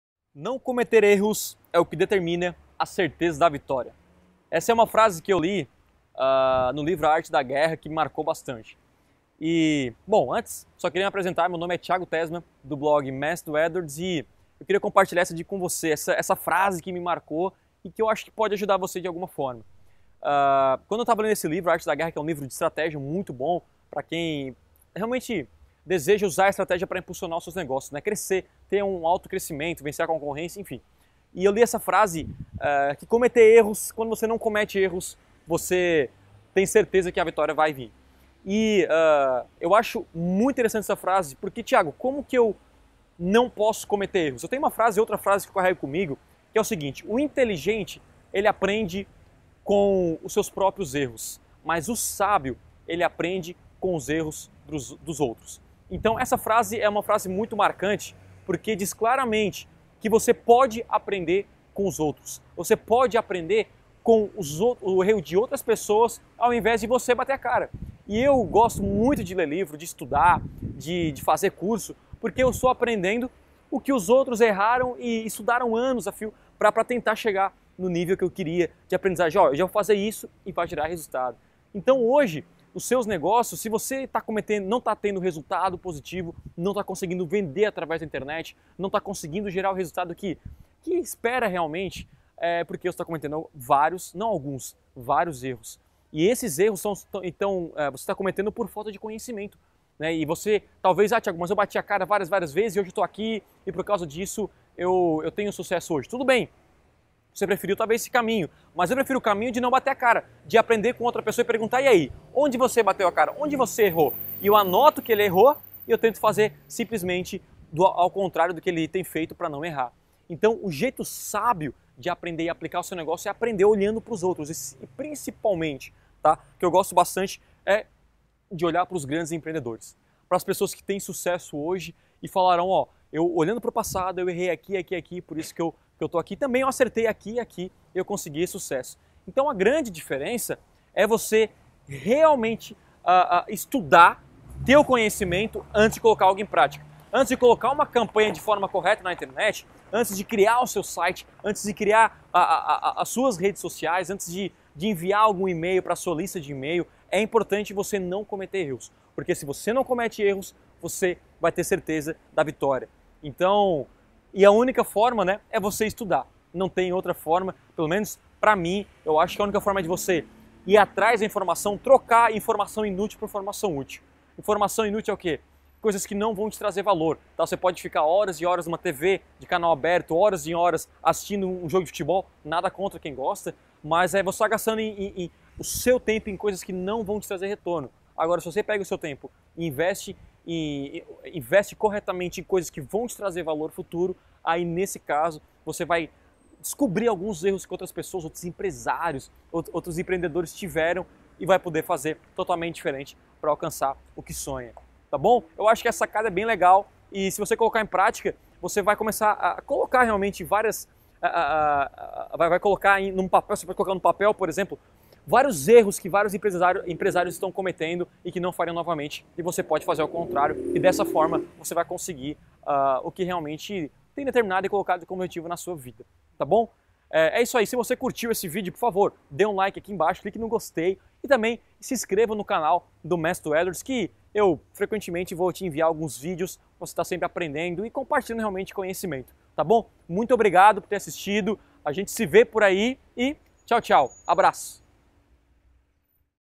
Não cometer erros é o que determina a certeza da vitória | Tiago Tessmann
Os sábios sempre aprendem com os erros dos outros enquanto os inteligentes aprendem com os próprios erros. Não cometer erros, ou o mínimo de erros que você cometer em seus negócios, maior a probabilidade de sucesso, e é por isso que a informação, networking e o estudo são fatores fundamentais para que diminua as chances de você errar.